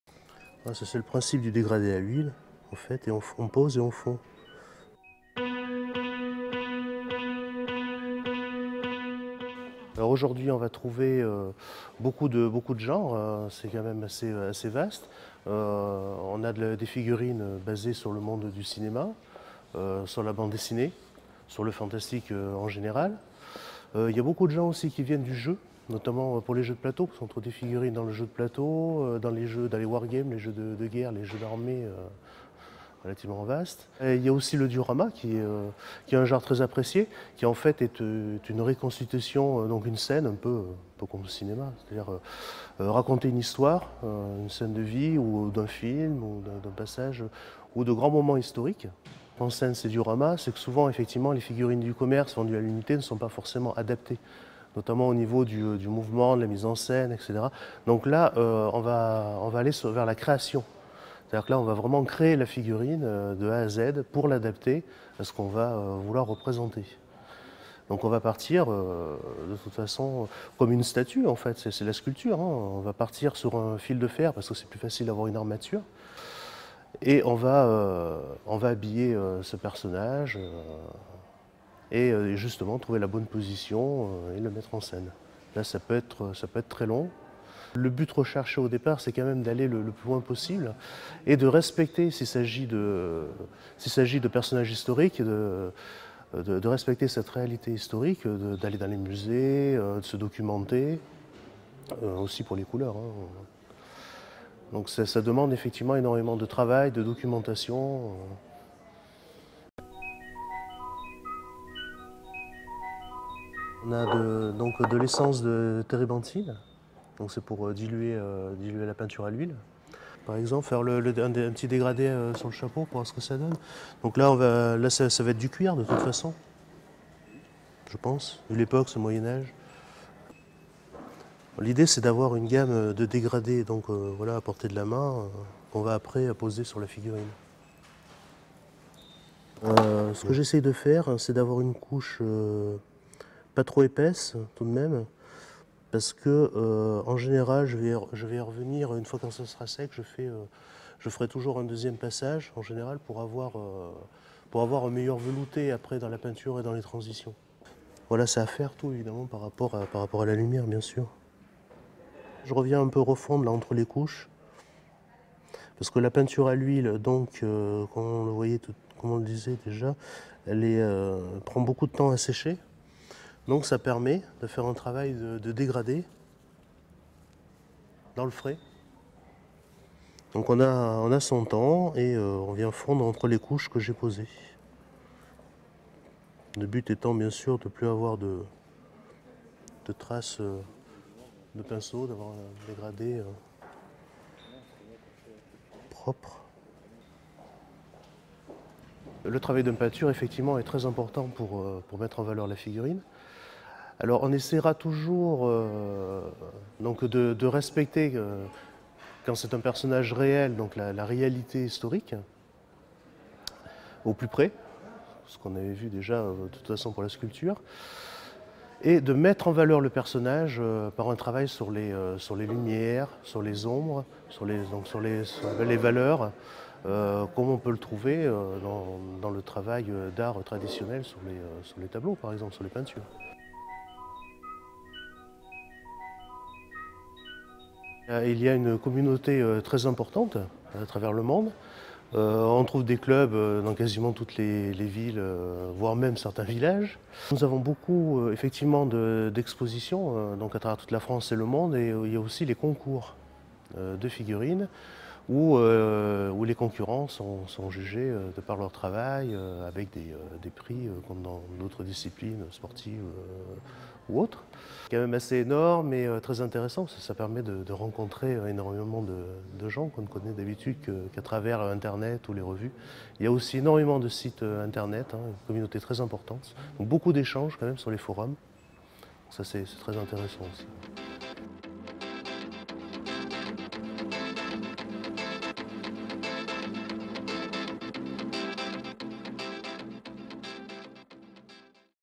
Exposition de figurines à la Bfm
Le club Jourdan présente l'exposition Figurine du 10 mars au 29 mars 2014 dans le Jardin d'hiver de la Bfm de Limoges Atelier pratiques les samedi 15, 22 et 29 mars à 14h